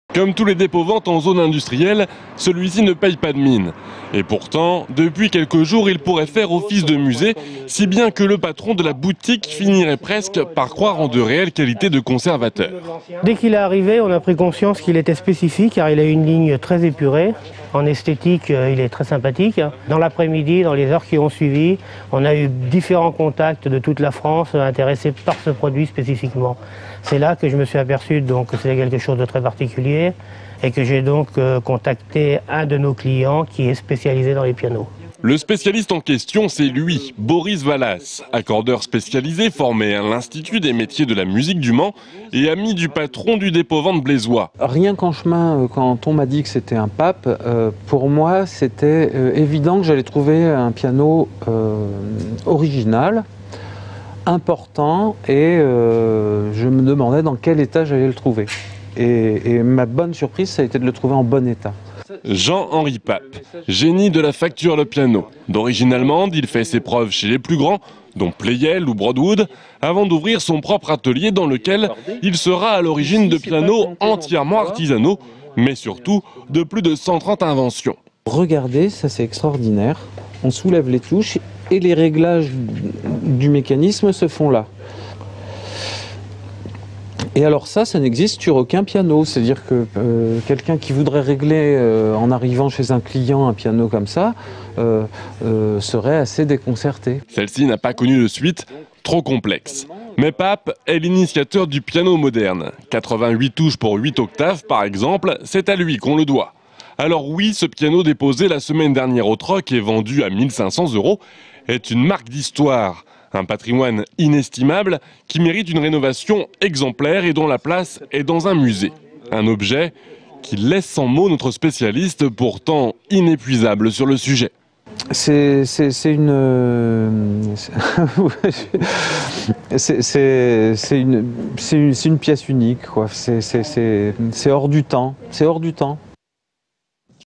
Un piano pape dans un dépôt vente de Blois!
Sites webs/blogs, copiez cette vidéo avec le bouton "Intégrer" !
 Un piano pape de 1839 était entreposé dans un dépôt vente de Blois.